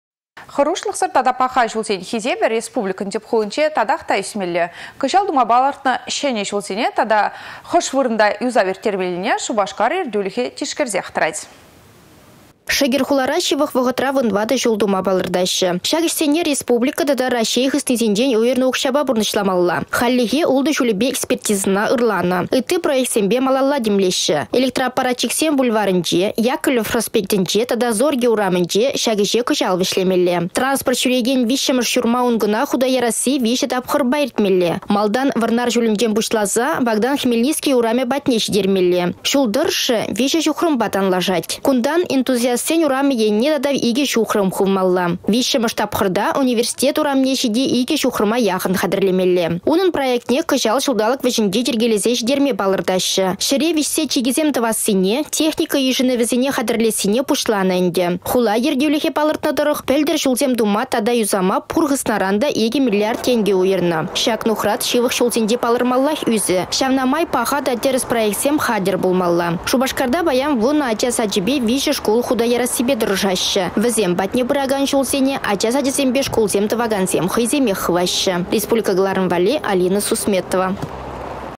Çĕнĕ тата юсанă çулсем
Хăрушлăхсăр тата паха çулсен хисепĕ республикăн тĕп хулинче татах та ӳсмелле. Кăçал тума палăртнă çĕнĕ çулсене тата хăш вырăнта юсав ирттермеллине Шупашкар ертӳлĕхĕ тишкерсех тăрать.